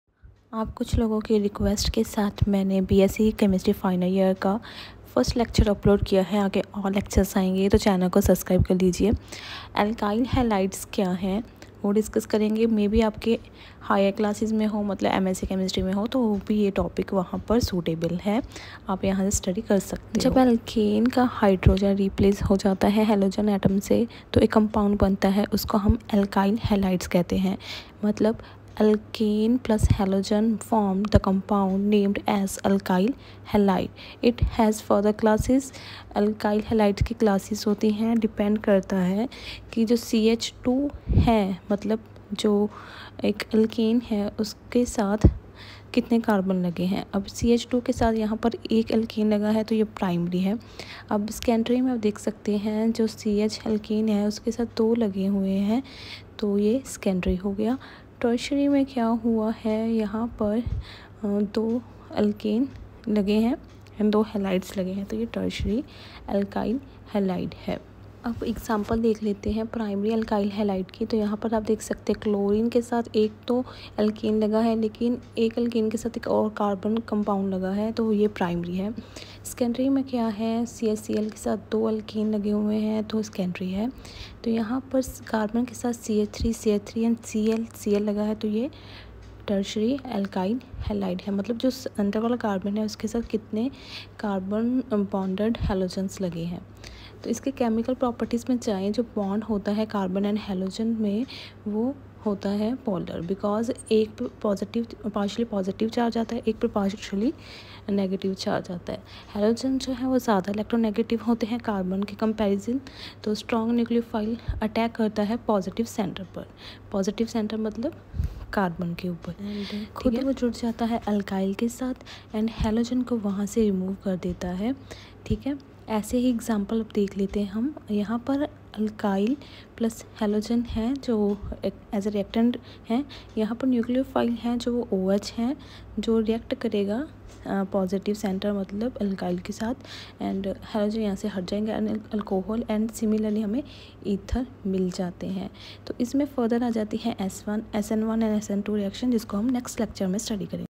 Alkyl halide ||Bsc chemistry final year notes reaction & reactivity of halogenated hydrocarbons
Alkyl halide ||Bsc final year|| handwritten notes reaction & reactivity of halogenated hydrocarbons Bsc_chemistry Bsc_final_year alkyl_halide. reaction_&_reactivity_of_halogenated_hydrocarbons chemistry.